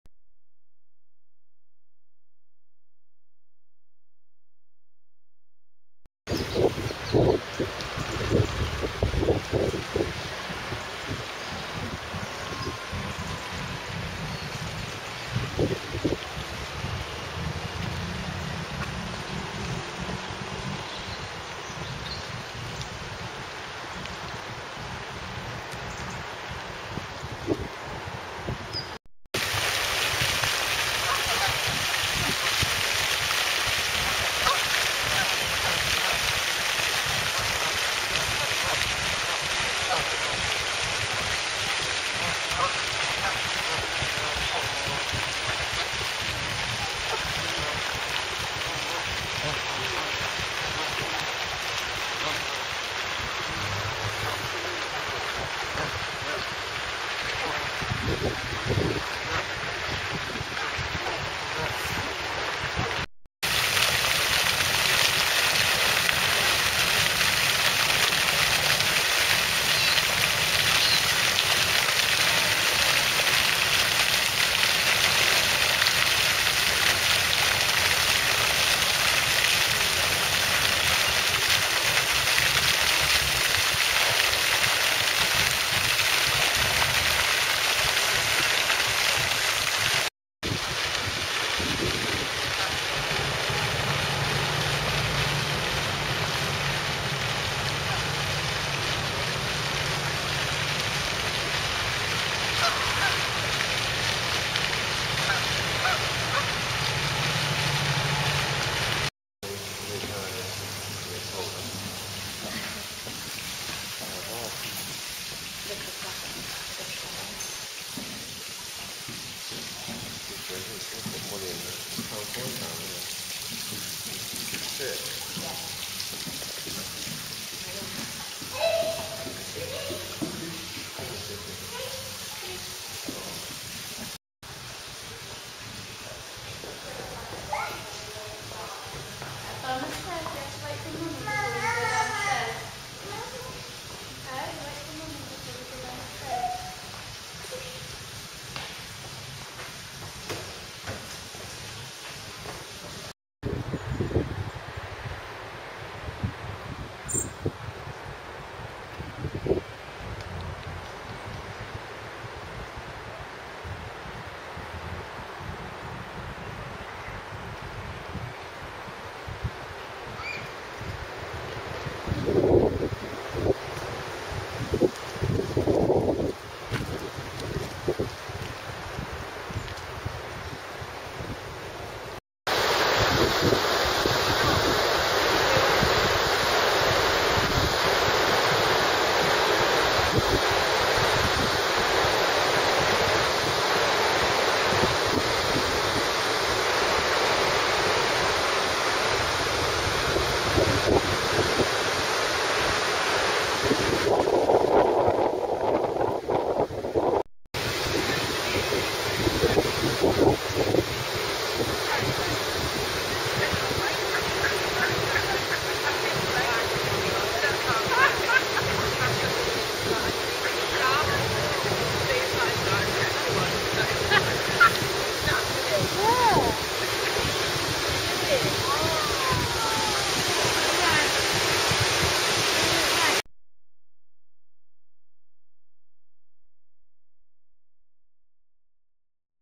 Jephson Gardens, Leamington Spa, Warwickshire
Video of Jephson Gardens, one of the best bits of central Leamington.